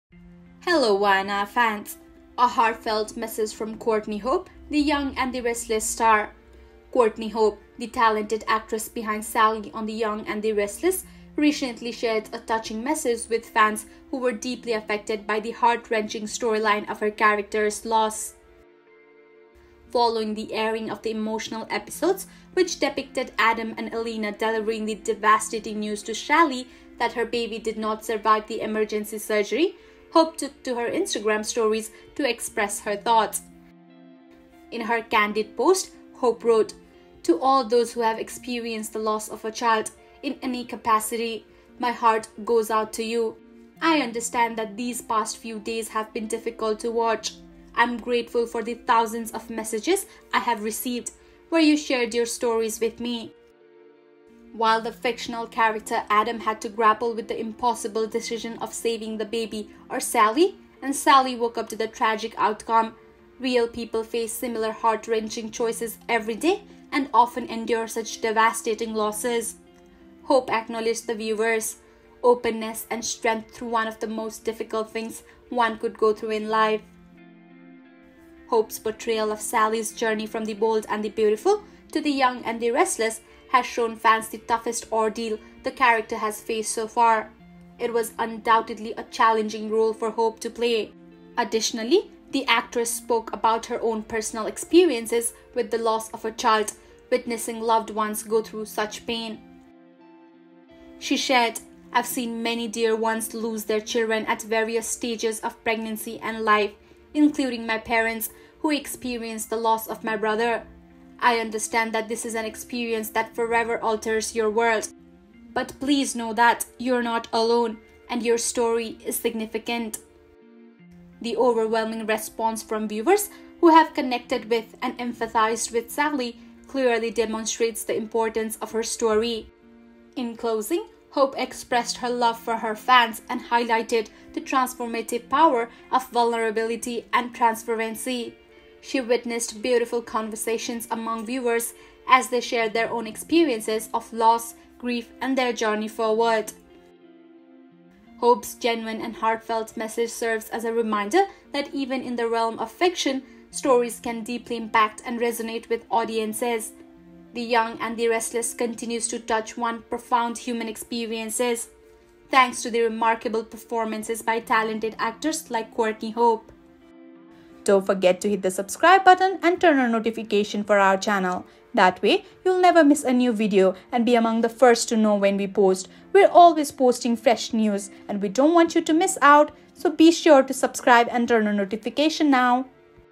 Young & Restless Casting Update: Courtney Hope is leaving the show!
A Heartfelt Message from Courtney Hope, the Young and Restless Star. Courtney Hope, the talented actress behind Sally on The Young and the Restless, recently shared a touching message with fans who were deeply affected by the heart-wrenching storyline of her character's loss. Following the airing of the emotional episodes, which depicted Adam and Elena delivering the devastating news to Sally that her baby did not survive the emergency surgery, Hope took to her Instagram stories to express her thoughts.

In her candid post, Hope wrote, "To all those who have experienced the loss of a child, in any capacity, my heart goes out to you. I understand that these past few days have been difficult to watch. I am grateful for the thousands of messages I have received, where you shared your stories with me."